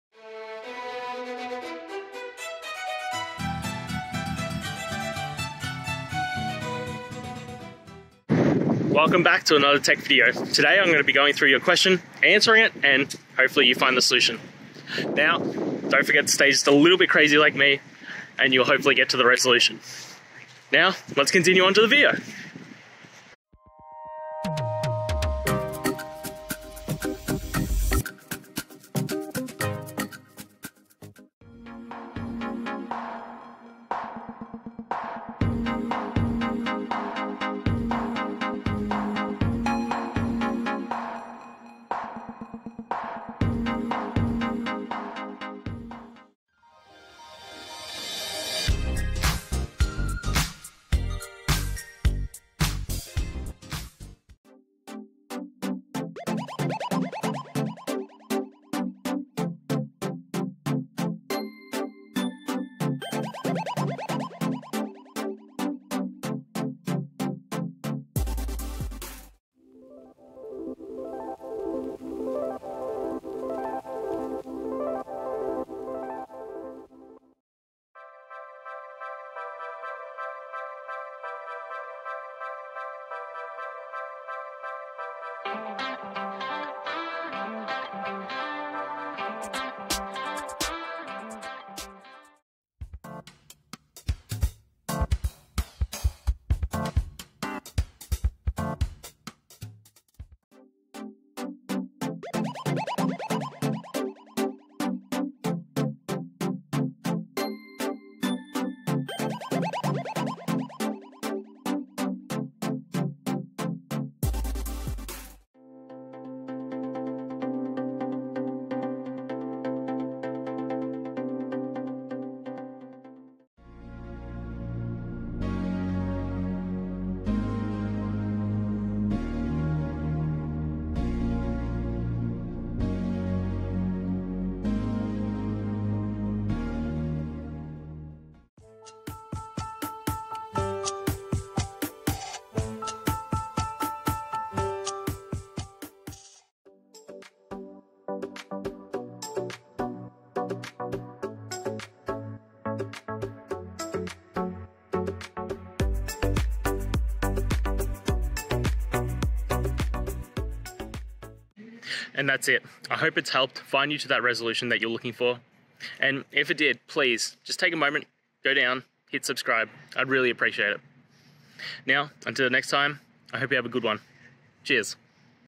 Why does Jenkins say "This Jenkins instance appears to be offline"
jenkins: Why does Jenkins say &quot;This Jenkins instance appears to be offline&quot;

Related to: jenkins, offline